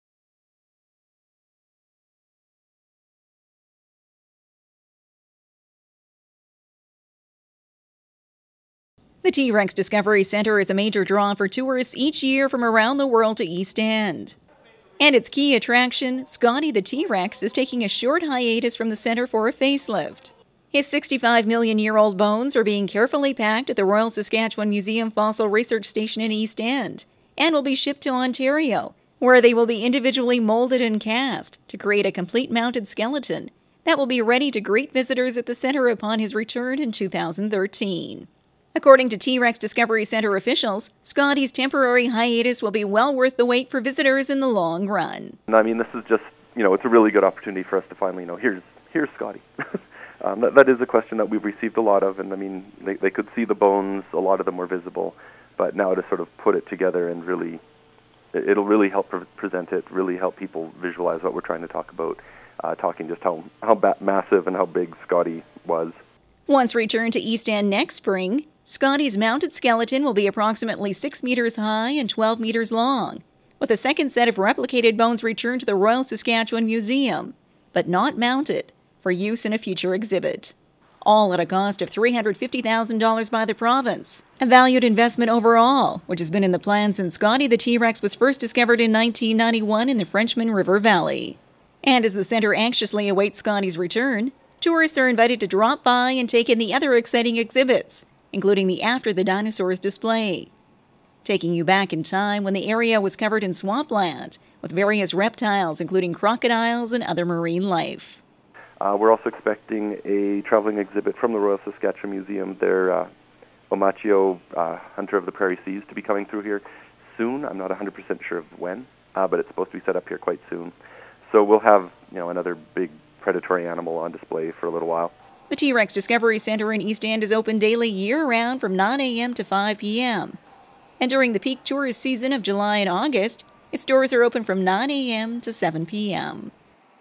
Scotty The Trex Gets Face Lift
Scotty the T. rex is temporarily leaving his home at the T.rex Discovery Centre in Eastend. His 65 million year old bones are being carefully packed at the Royal Saskatchewan Museum Fossil Research Station in Eastend and will be shipped to Ontario, where they will be individually molded and cast to create a complete mounted skeleton that will be ready to greet visitors at the centre upon his return. Two sets of replica bones are being cast from the original fossils. One will be returned to the T.rex Centre as a mounted skeleton in time for the busy tourist season of 2013. The skeleton will be approximately six metres tall and more than 12 metres long. A second set of replicated bones will be returned to the Royal Saskatchewan Museum, but not mounted, for use in a future exhibit. The Province is investing $350 000 towards the Scotty project.

Although Scotty will be absent from the T.rex Discovery Centre until 2013, tourists are encouraged to still drop by the facility to view the other exciting exhibits. Current Hours of Operation are 9-5pm then and 9am-7pm during July and August. SWTV News File Image.